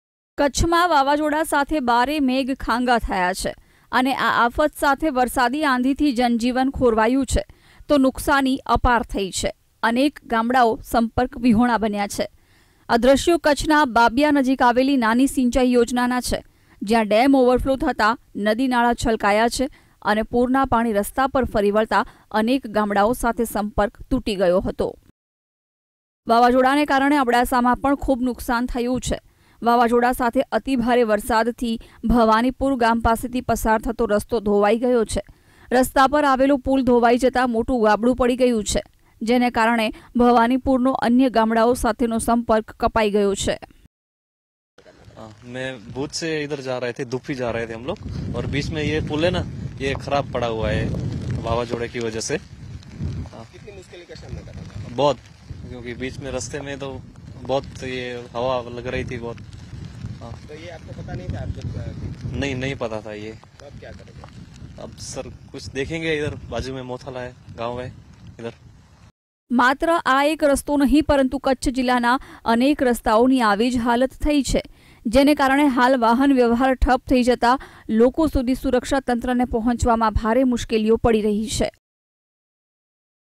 Biporjoy Cyclone:આંધી સાથે વરસાદ, ધોધમાર વરસાદથી છલકાયાં નદી-નાળા, અબડાસા તાલુકામાં રોડ-રસ્તા ધોવાયા
બિપોરજોય લાવ્યું આંધી સાથે વરસાદ, ધોધમાર વરસાદથી છલકાયાં નદી-નાળા, અબડાસા તાલુકામાં રોડ-રસ્તા ધોવાયા